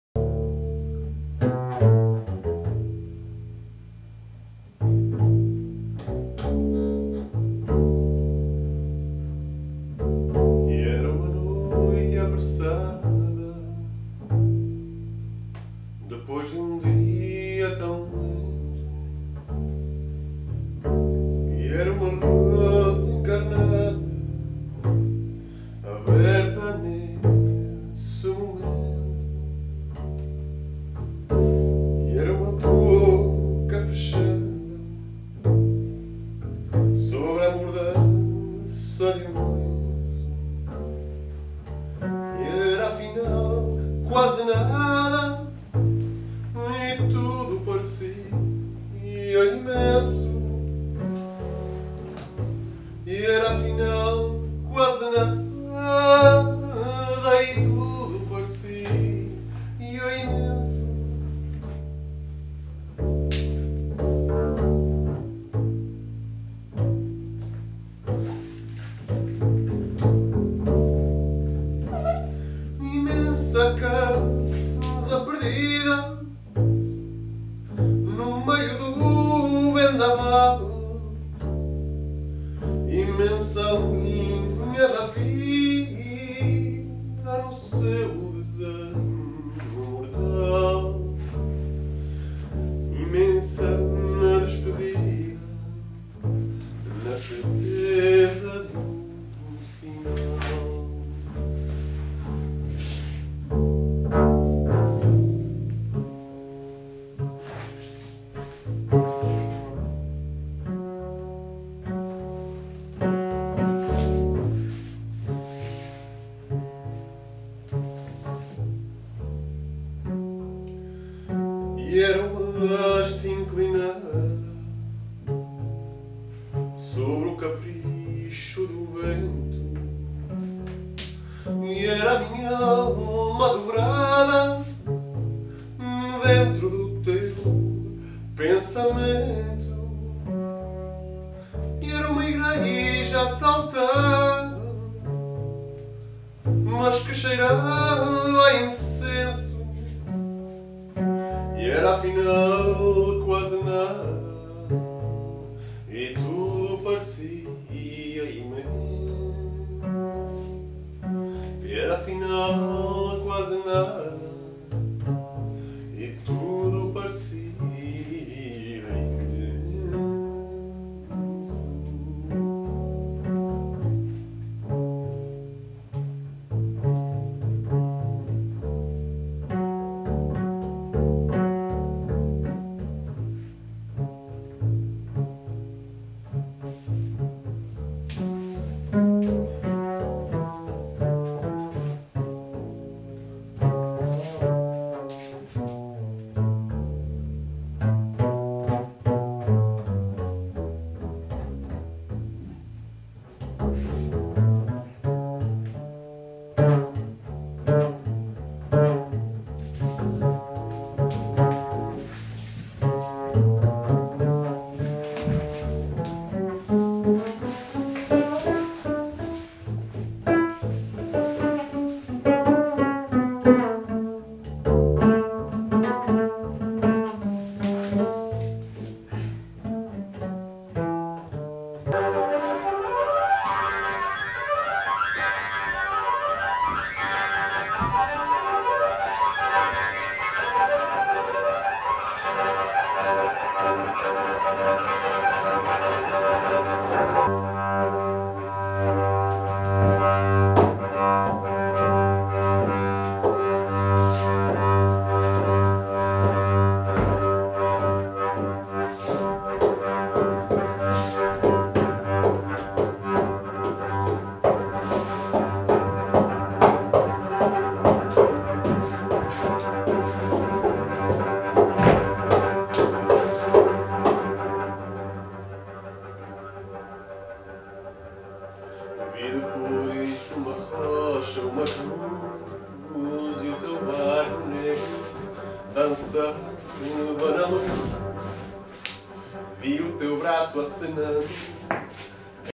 FadoProject
Rehearsal/Improvisation feature by Rui Salgado and Marta Kosieradzka